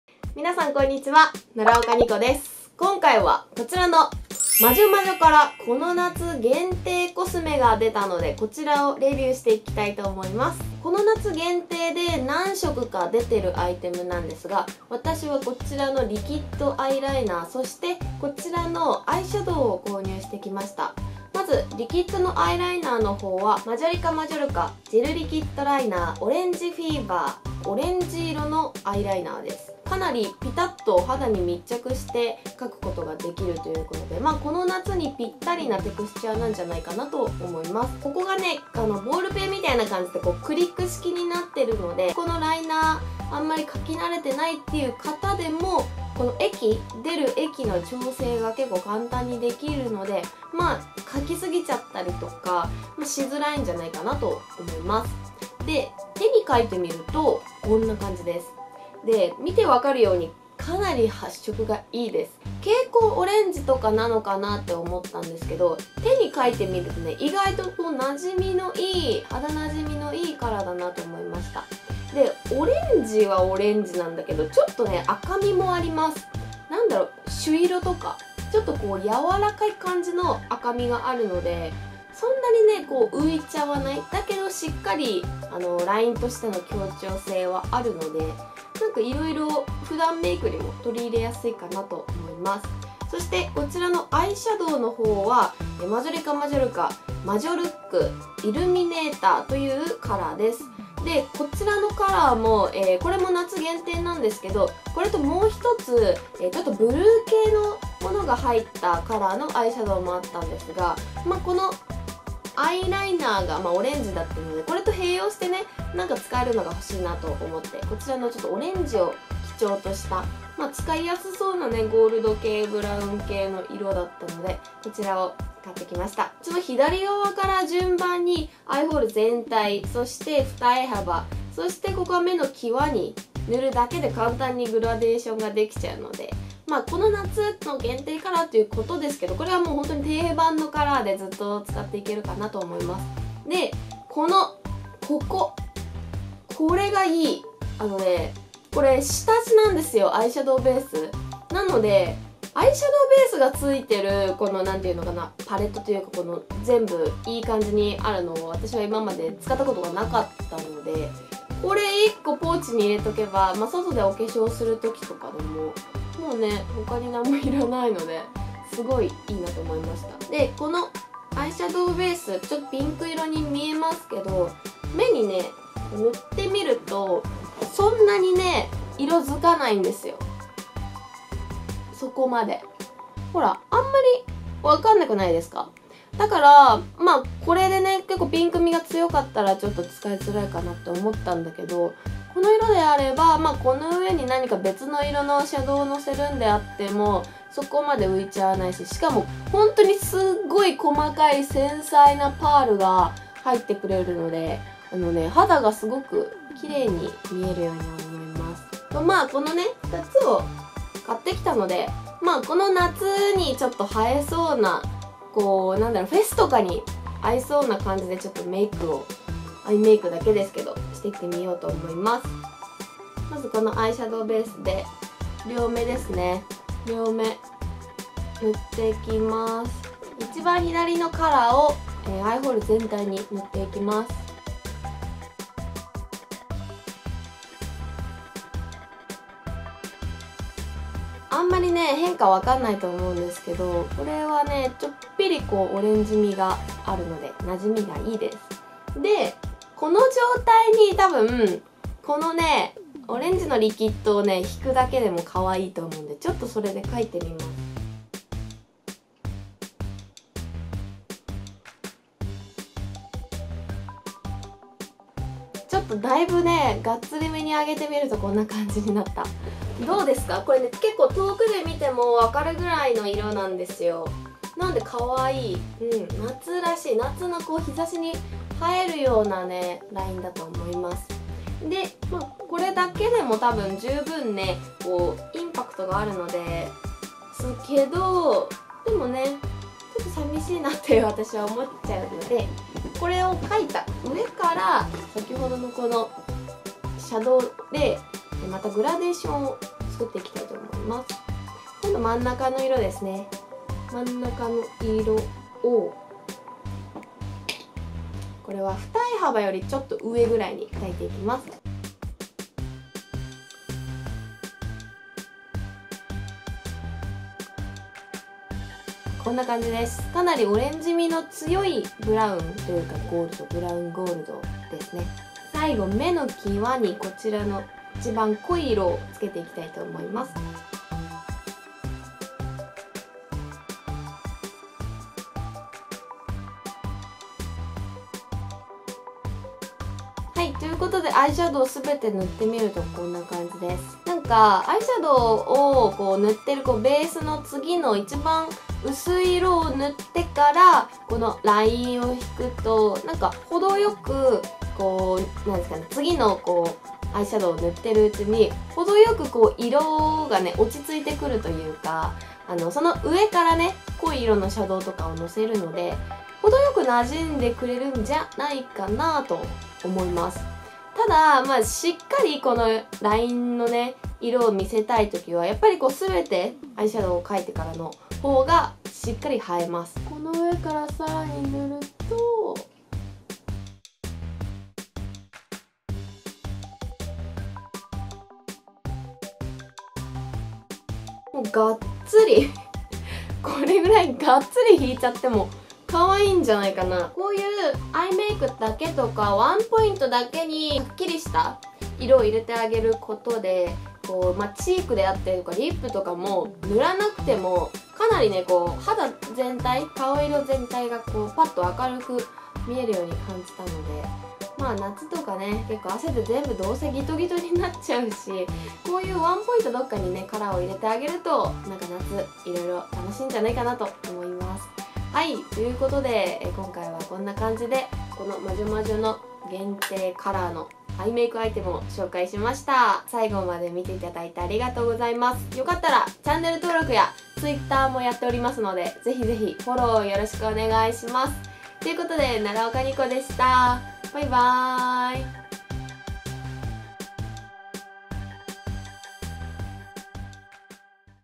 夏の日差しに映えるメイク♡ マジョマジョ夏の新色アイラインとアイシャドウが可愛い！
奈良岡にこチャンネルは、毎週水・土曜日の19時に動画UP！